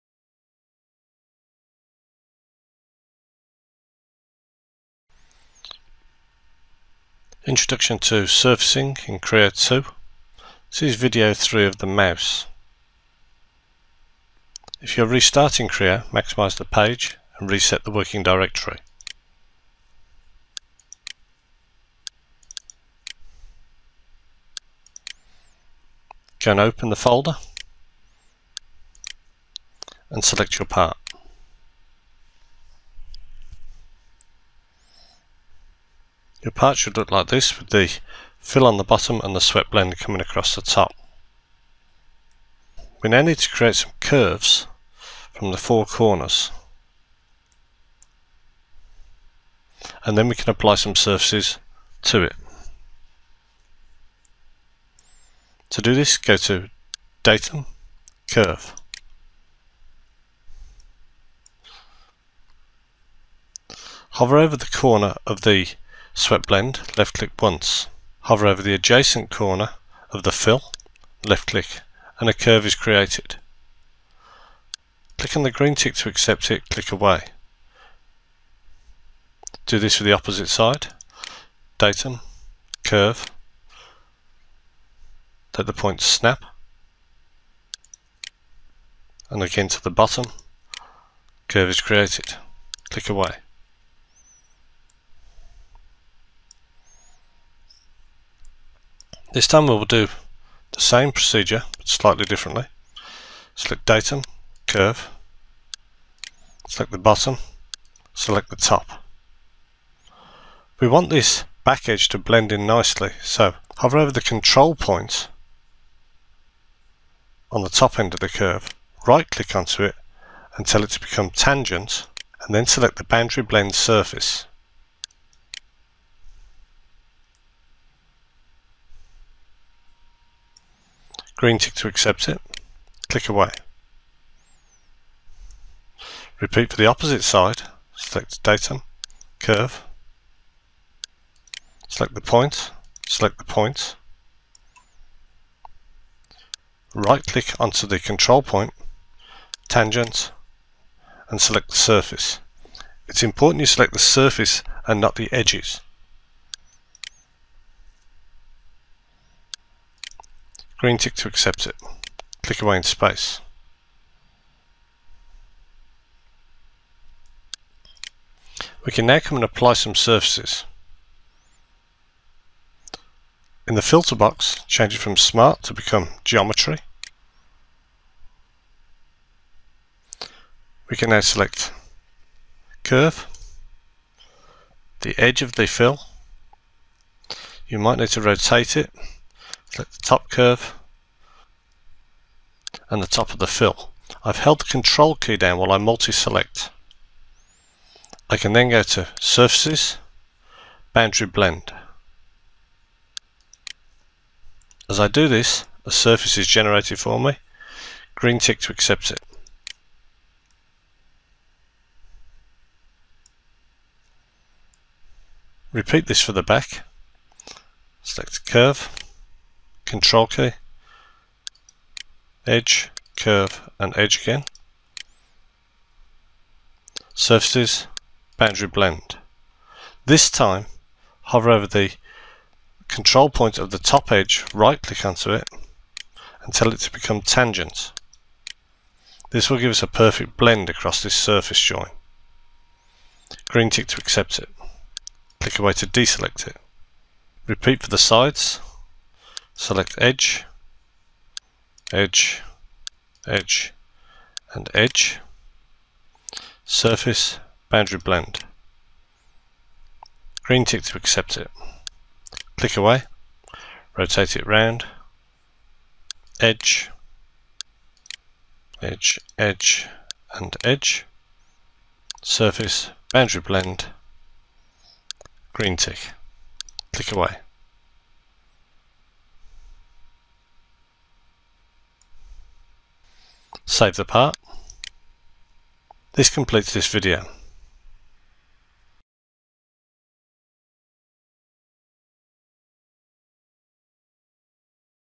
Creo 2.0 CAD Tutorial ( AUEA) Surfacing Video 3
This is the 3rd Video in the series.
This Video covers completing the Curves and then fitting the Surfaces